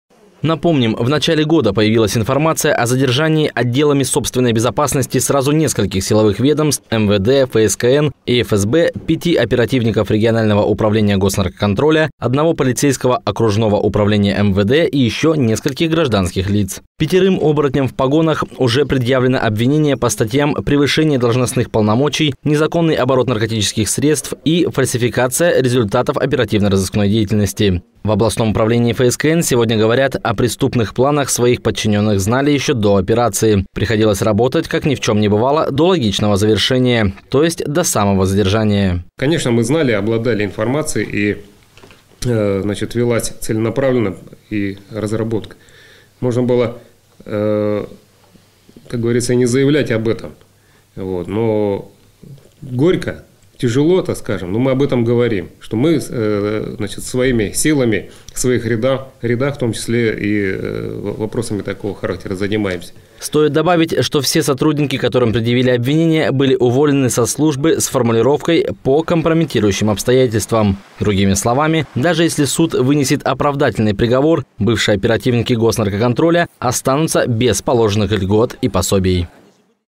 Комментарий УФСКН по громкому уголовному делу
В региональном управлении Госнаркоконтроля знали об оперативной разработке группы "оборотней в погонах", которую задержали в начале этого года. В ведомстве, наконец, прокомментировали самое громкое уголовное дело в отношении силовиков за последние несколько лет.